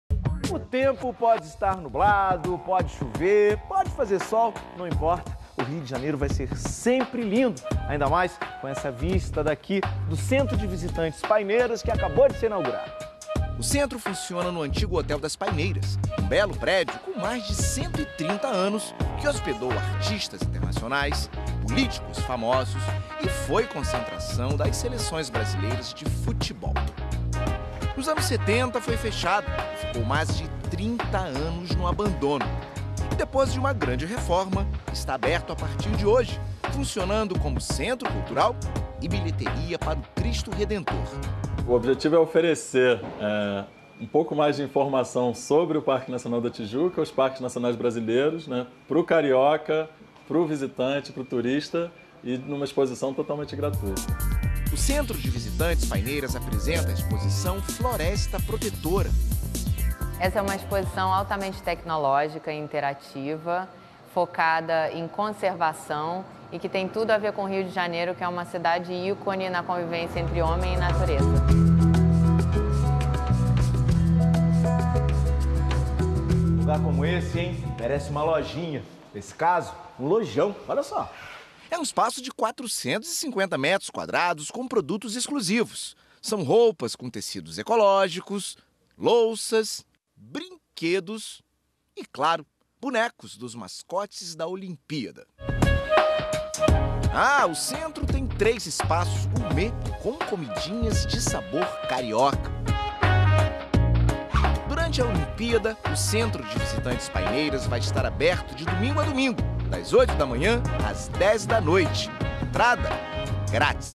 Hotel Paineiras
01/08/2016
Vídeo sobre a inauguração do Centro de Visitantes das Paineiras: projetos comerciais por Kube Arquitetura (loja, quiosques e restaurante)